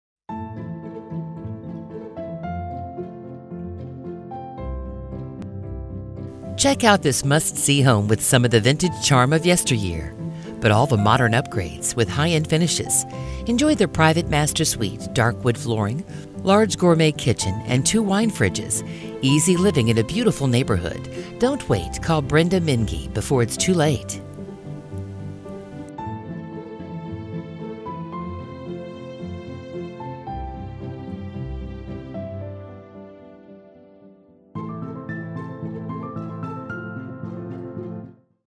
4927 N Pennsylvania Street Indianapolis, IN | MLS# 21726466
4927 N Pennsylvania Street, Indianapolis, IN

Price reduced! Come see this Modern Farmhouse beauty in Meridian-Kessler! A must-see with some of the vintage charm of yesteryear, but all of the modern upgrades and end finishes - private master suite, dark wood flooring, large gourmet kitchen, 2 wine fridges, a roof top deck to enjoy on those cool